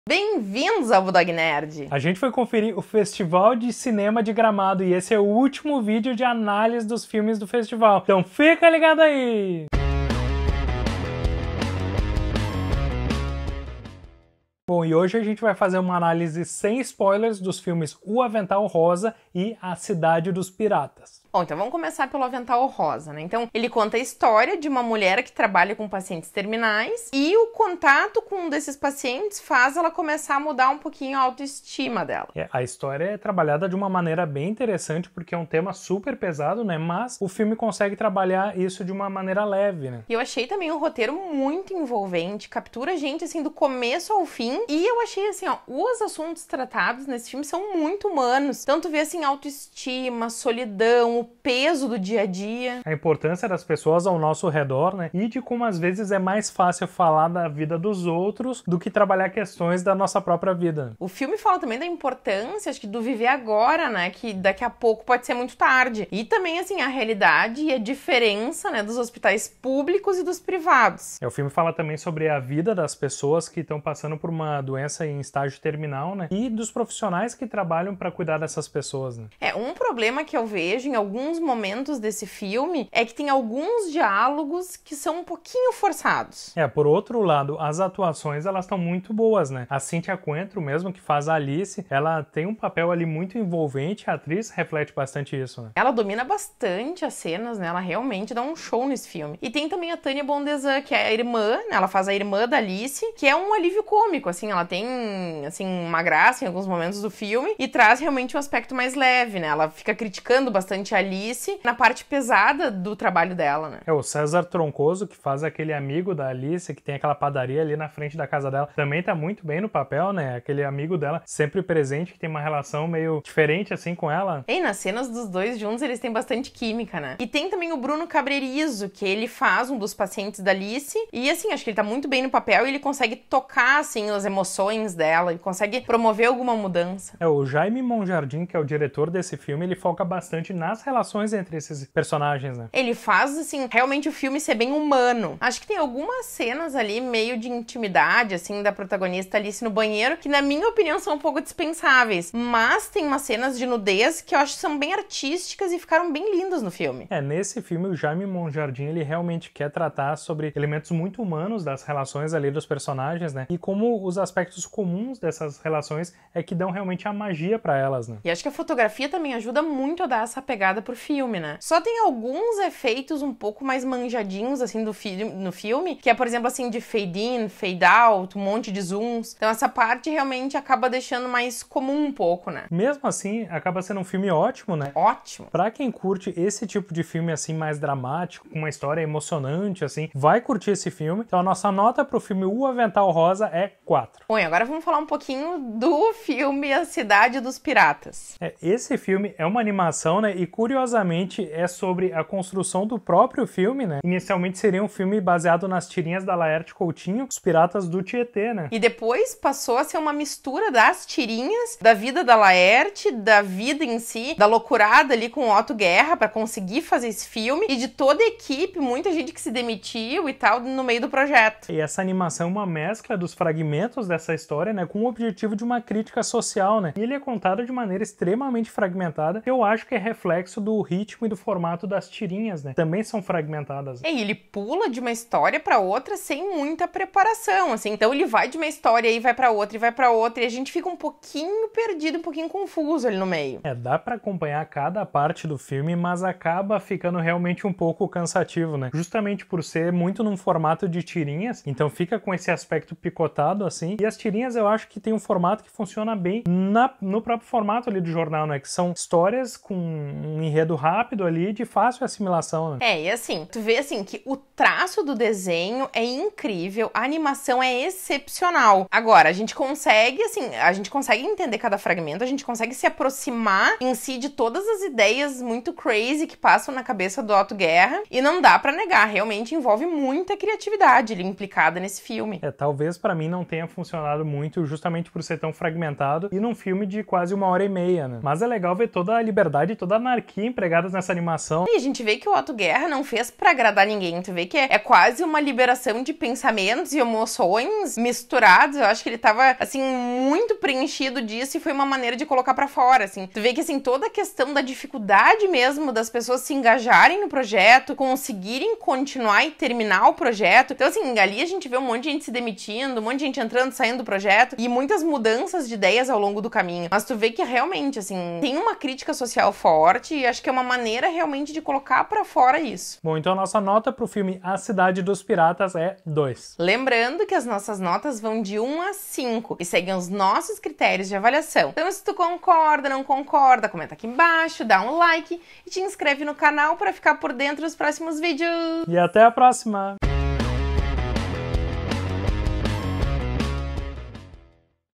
Análise sem spoilers dos filmes O AVENTAL ROSA / A CIDADE DOS PIRATAS
Análise sem spoilers dos filmes "O Avental Rosa" e "A Cidade dos Piratas", que integram o 46º FESTIVAL de CINEMA de GRAMADO.